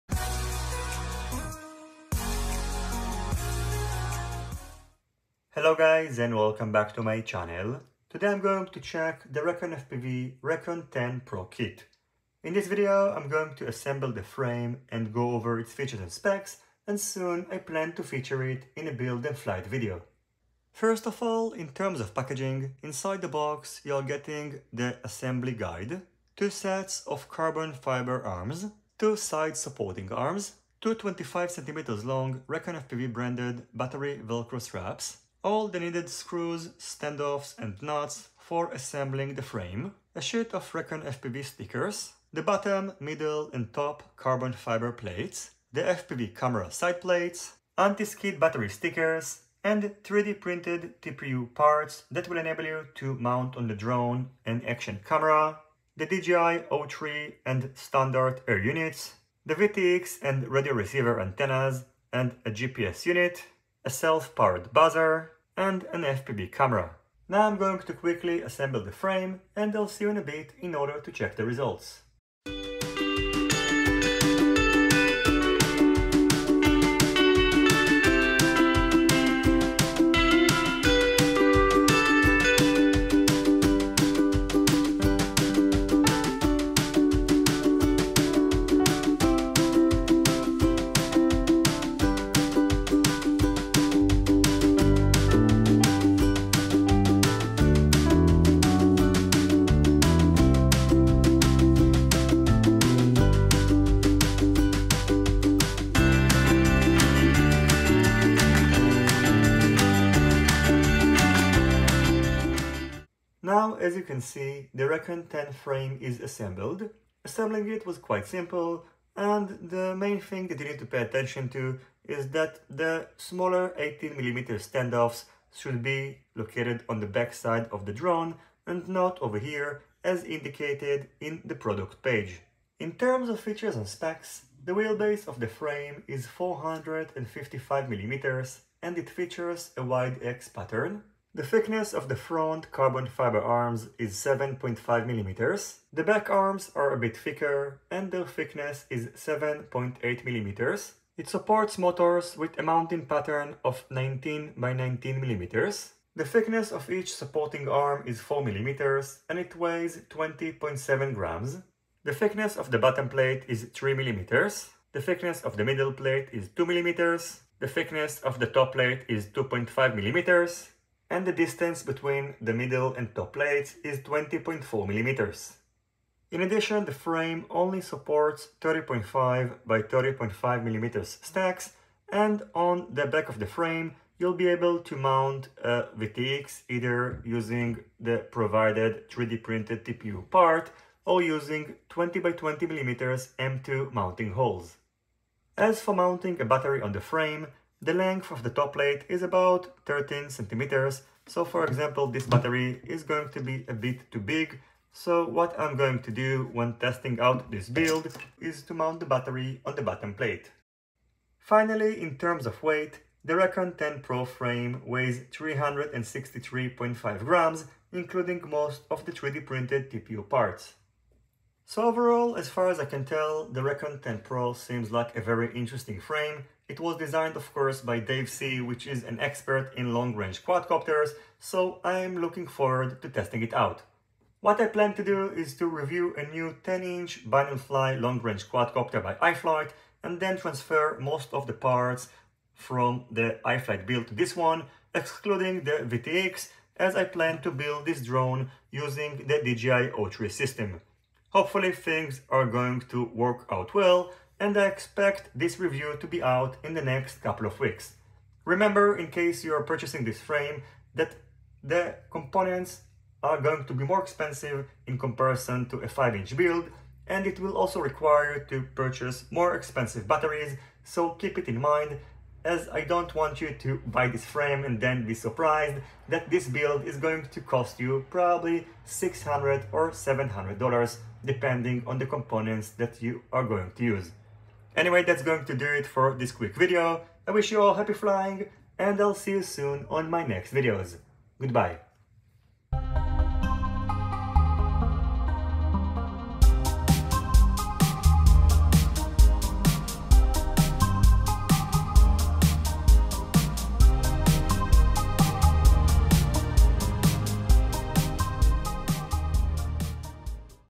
RekonFPV Rekon10 PRO Long Range Frame Kit - Build & Overview
╠╗║╚╝║║╠╗║╚╣║║║║║═╣ I Really Appreciate Your Support!

Description

The Rekon 10 Pro is an absolute beast designed for the most challenging long-range missions! The weight and momentum of a 10” quad gives it stunningly high cruising speeds and wind resistance for high altitude mountain dives. While it looks similar to the Rekon 7 Pro, they actually not share any identical carbon parts:
The Arms are considerably beefed-up and cut out of 8mm carbon plates.
Top and bottom plate are 3mm thick.
The main body is wider and slightly longer to fit larger batteries.
For maximum range dual batteries (top and bottom mount) are recommended.

Rekon10 PRO Long Range Frame Kit 10 Inch for FPV Racing Drone

Product name:Rekon
Model:Rekon10 PRO LR Frame kit
Material:3K Carbon fiber
Type:10 inch
Wheelbase:455mm
Size:476 * 298mm
Top plate thickness:3mm
Bottom plate thickness:3mm
Middle plate thickness:2mm
Front arm:8mm
Rear arm:8mm
Frame protection bar:2mm
Camera side plate:2mm
Top and middle plate space:20mm
Flight controller mounting hole:30.5x30.5mm
Camera mount:20*20mm
Motor mounting hole:19*19mm
Weight:396.7g

Package includes:
1x 3mm top plate
1x 3mm base plate
1x 2mm mid plate
2x 8mm front arm
2x 8mm rear arm
2x 2mm rack protection board
2x 2mm camera side panel
2x M3*30mm black mesh aluminum column
4x M3*20mm black mesh aluminum column
2x M3*18mm black mesh aluminum pillar
4x M4*16 Screws
4x M3*22 Screws
4x M3*20 Screws
4x M3*10 Screws
8x M3*8 Screws
4x M2*20 Screws
1x M2*16 Screws
2x M2*4 Screws
1x M5 lock nut (red)
4x M3 self-locking nuts
5x M2 self-locking nuts
8x M3*8 shock-absorption balls
4x M2*4 shock-absorption balls
2x battery straps 20x250mm
1x Battery Anti-slip pad
1x Down Battery Anti-slip pad
1x TPU black GoPro holder mount
1x TPU black GPS mount
2x TPU black antenna mount
1x TPU black T-antenna mount
1x TPU black buzzer mount
1x TPU black DJI Air unit Block A
1x TPU Black DJI Air unit Block B
1x TPU black DJI O3 holder mount

Recommended parts:(not included)
Flight control:Zeus F722
Motor:31xx/32xx
ESC:60A
Camera:20X20mm
Propeller:10 inches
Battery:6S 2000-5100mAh